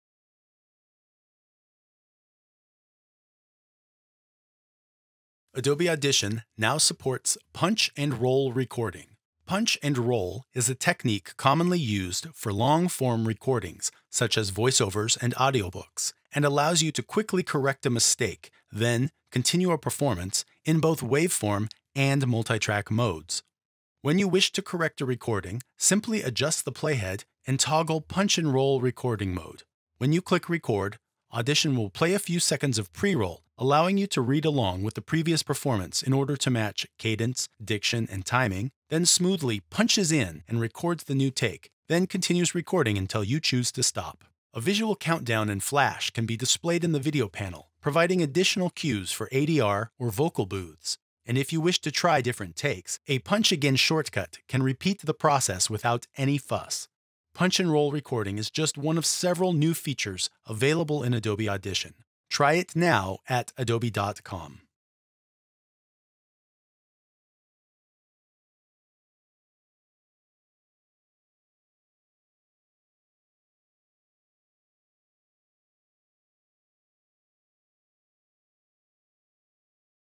Punch and Roll Recording in Audition April 2019 | Adobe Creative Cloud
Punch and Roll recording enables quick correction of mistakes, without interrupting flow for long-form recordings, such as narration, voiceovers, and audiobooks. Pre-roll playback and visual countdown helps your voice talent match the cadence of earlier performances, and quick commands enable multiple takes without additional fuss.